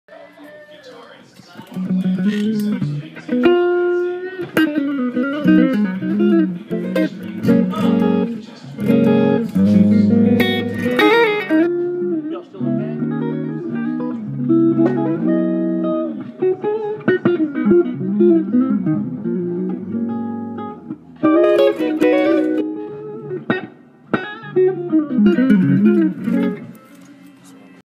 [Gospel Guitar] Church Licks
Some quick riffs I use when playing behind a preacher.